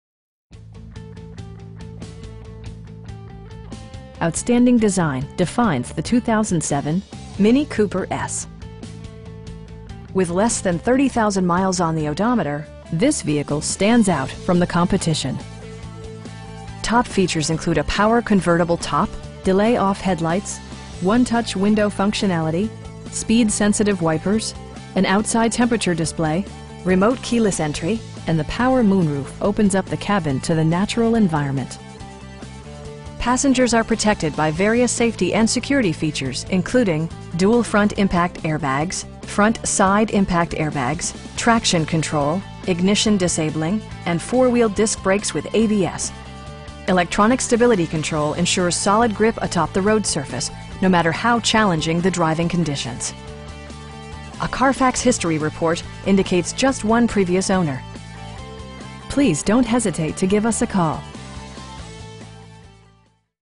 2007 MINI Cooper S Mini in Pembroke Pines, FL 33027
Subaru of Pembroke Pines

Outstanding design defines the 2007 MINI Cooper S. With less than 30,000 miles on the odometer, this vehicle stands out from the competition! Top features include a power convertible top, delay-off headlights, 1-touch window functionality, speed sensitive wipers, an outside temperature display, power windows, remote keyless entry, and the power moon roof opens up the cabin to the natural environment. Passengers are protected by various safety and security features, including: dual front impact airbags, front SIDE impact airbags, traction control, ignition disabling, and 4 wheel disc brakes with ABS. Electronic stability control ensures solid grip atop the road surface, no matter how challenging the driving conditions. A Carfax history report indicates just one previous owner. We offer competitive pricing, and the area's best shopping experience. Please don't hesitate to give us a call.